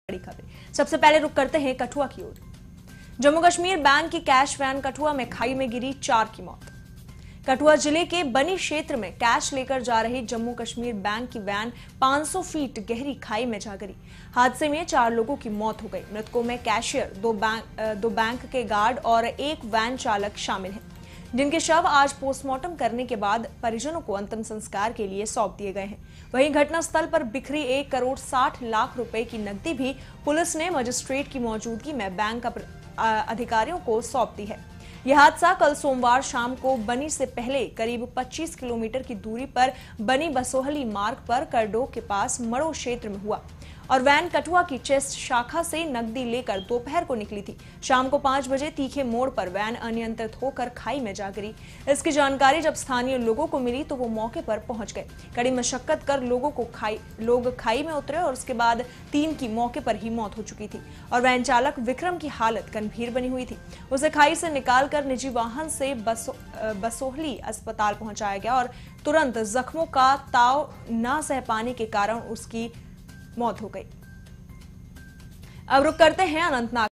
kathua जम्मू-कश्मीर बैंक की कैश वैन कठुआ में खाई में गिरी, चार की मौत
कठुआ जिले के बनी क्षेत्र में कैश लेकर जा रही जम्मू कश्मीर बैंक की वैन 500 फीट गहरी खाई में जा गिरी। हादसे में चार लोगों की मौत हो गई। मृतकों में कैशियर, दो गार्ड और एक वैन चालक शामिल है। जिनके शव आज पोस्टमार्टम करवाने के बाद परिजनों को अंतिम संस्कार के लिए सौंप दिए गए हैं। वहीं घटना स्थल पर बिखरी एक करोड़ साठ लाख रुपये की नकदी भी पुलिस ने मजिस्ट्रेट की मौजूदगी में बैंक अधिकारियोंको सौंप दी है। यह हादसा कल सोमवार शाम बनी से पहले करीब 25 किलोमीटर की दूरी पर बनी-बसोहली मार्ग पर करडोह के पास मड़ो क्षेत्र में हुआ। वैन कठुआ की चेस्ट शाखा से नकदी लेकर दोपहर को निकली थी। शाम पांच बजे तीखे मोड़ पर वैन अनियंत्रित होकर खाई में जा गिरी। इसकी जानकारी जब स्थानीय लोगों को मिली तो वे मौके पर पहुंच गए। कड़ी मशक्कत कर लोग खाई में उतरे। तीन की तो मौके पर ही मौत हो चुकी थी। वैन चालक विक्रम की हालत गंभीर बनी हुई थी। उसे खाई से निकाल निजी वाहन से बसोहली अस्पताल पहुंचाया गया। परंतु जख्मों का ताव न सह पाने के कारण उसने भी बाद में दम तोड़ दिया।